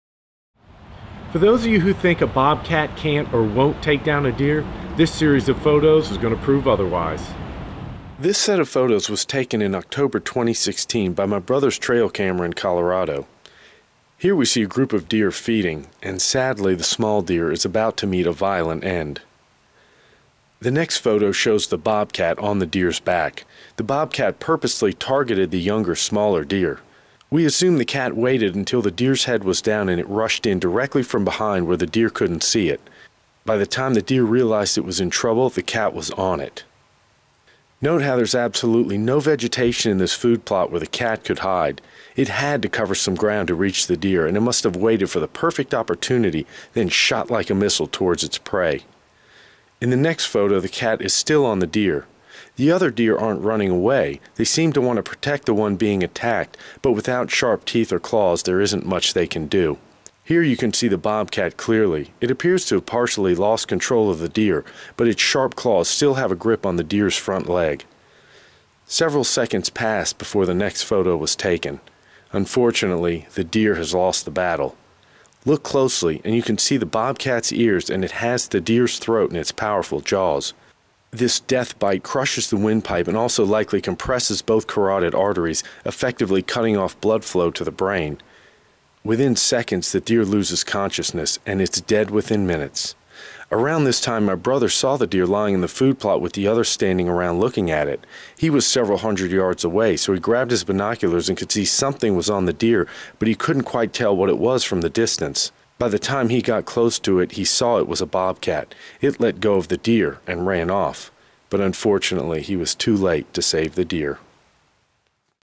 Bobcat Attacks and Kills a Deer (HD)
This dramatic series of photos shows a bobcat attack and kill a small mule deer.  The photos were taken in October, 2016 by my brother's trail camera.